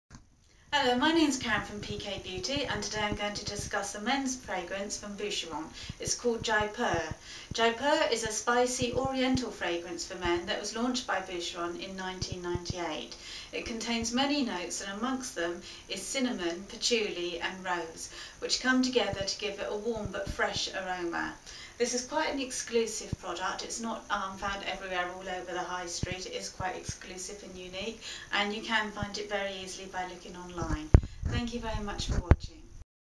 Boucheron Jaipur A Warm Spicy Fragrance for Men launched 1998
Karen from Pique Beauty discusses Boucheron Jaipur Fragrance for Him